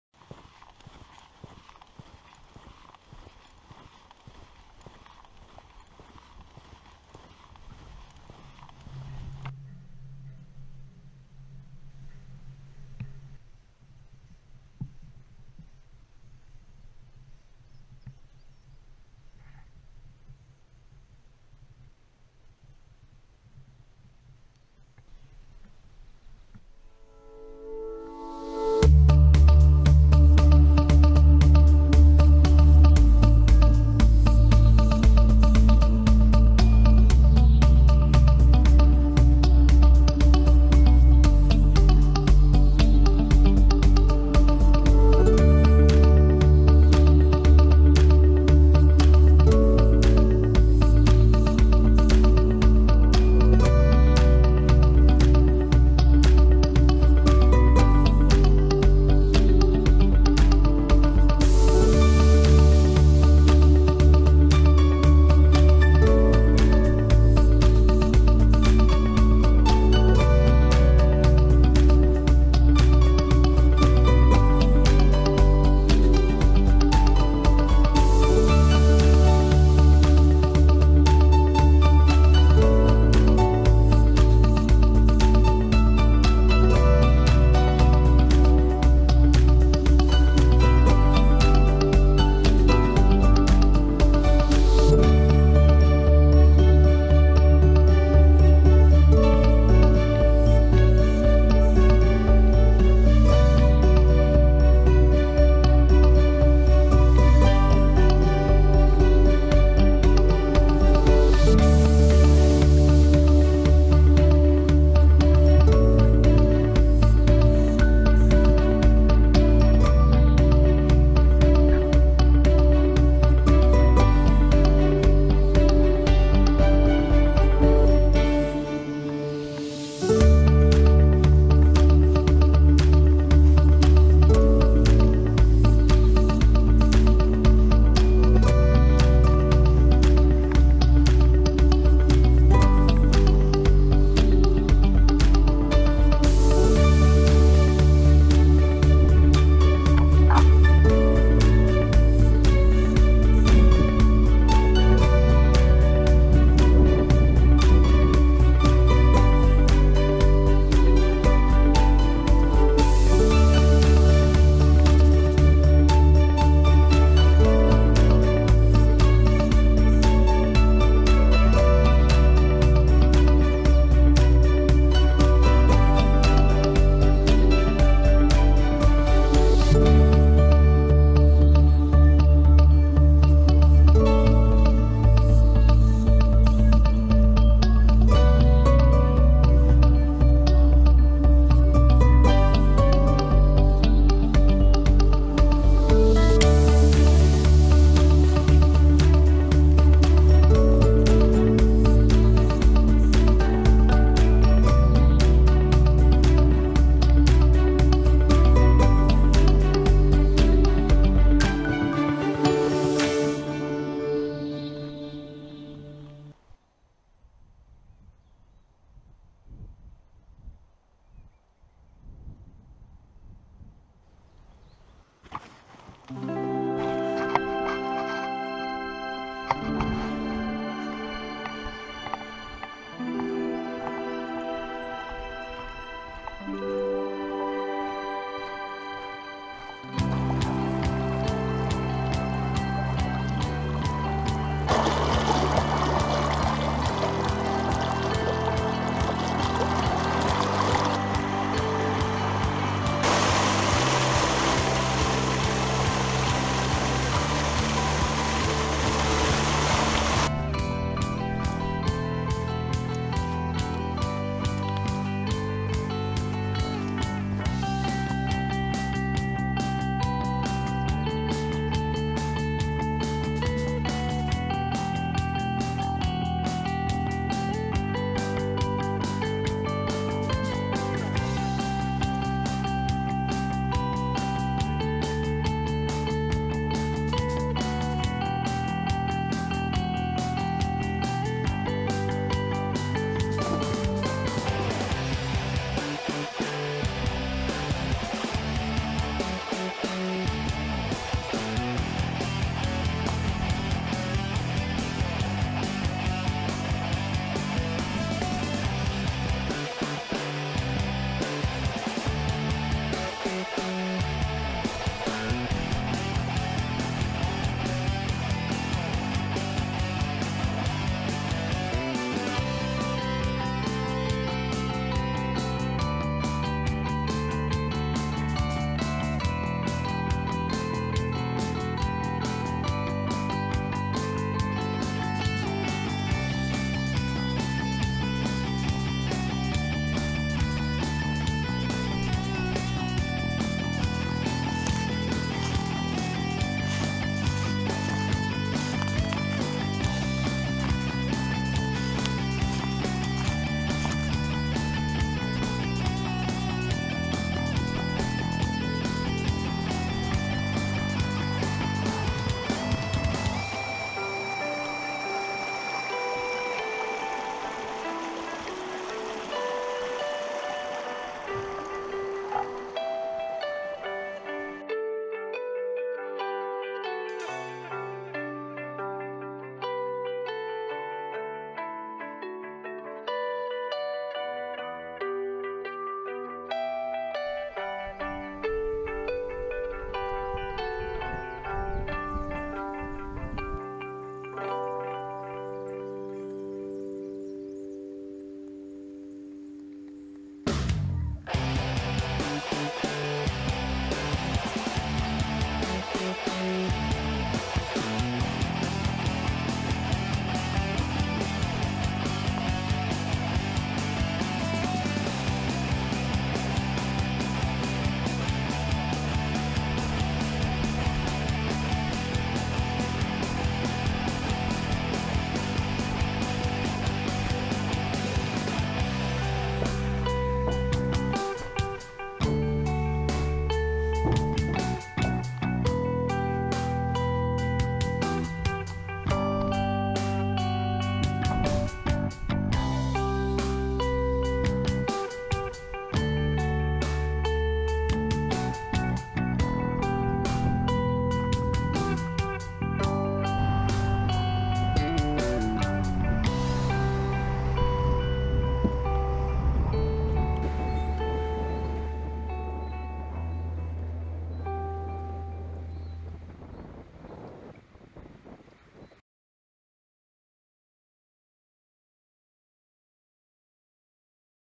Nord-Süd-Weitwanderweg 05: 4 Tage durchs Waldviertel - südwärts Kapitel 2
Am Weitwanderweg 05 vom Nebelstein zur Donau